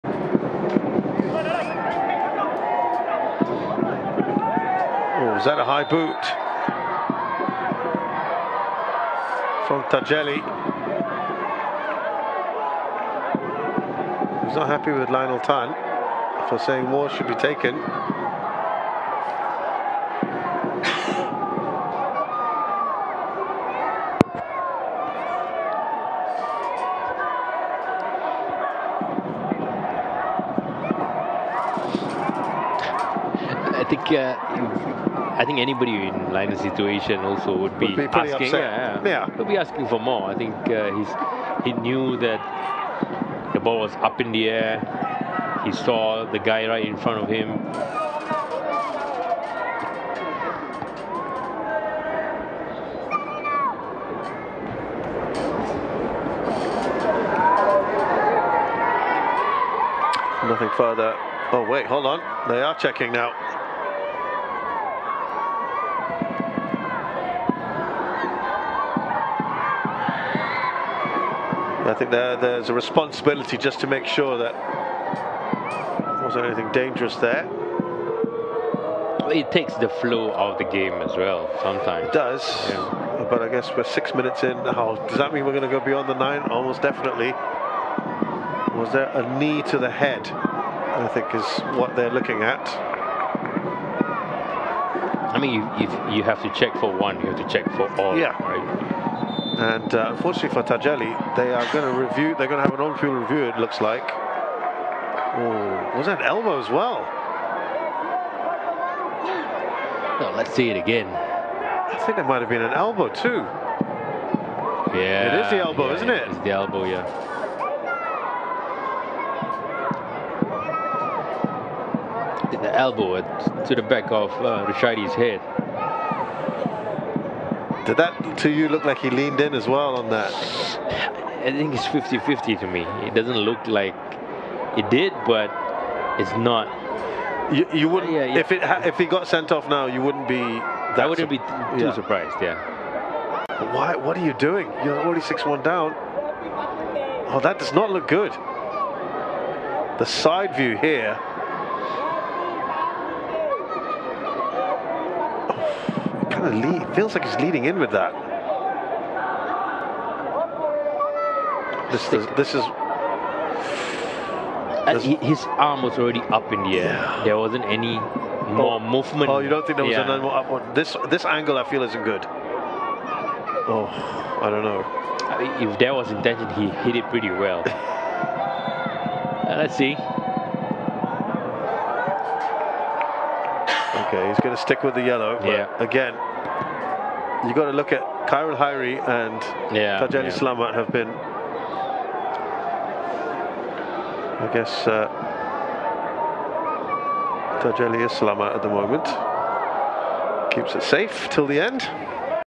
Tanjong Pagar's Tajeli escapes with a yellow after 1-2 combo hit on Sailors' Rusyaidi | SPL 2023
Rusyaidi Salime suffered blows to his head after a challenge by Tajeli Salamat. The referee initially showed a yellow card and stuck by his decision after VAR.